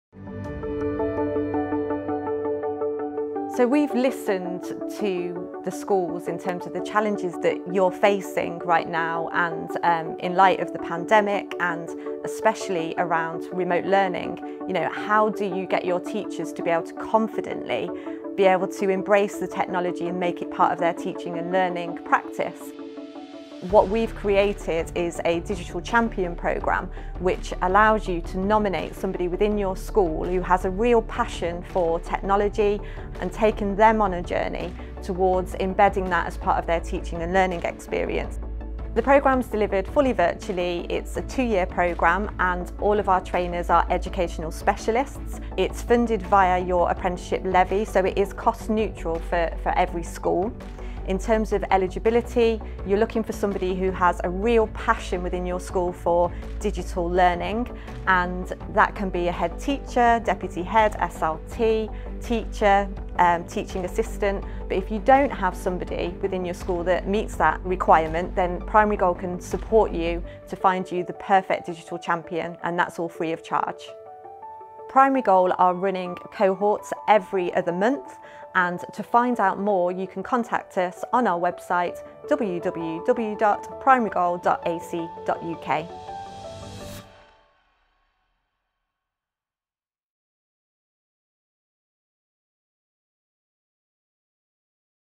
Primary Goal Digital Champion
Do you needed support to deliver your digital strategy in school? Primary Goal has created the Digital Champion training programme as a fully funded solution.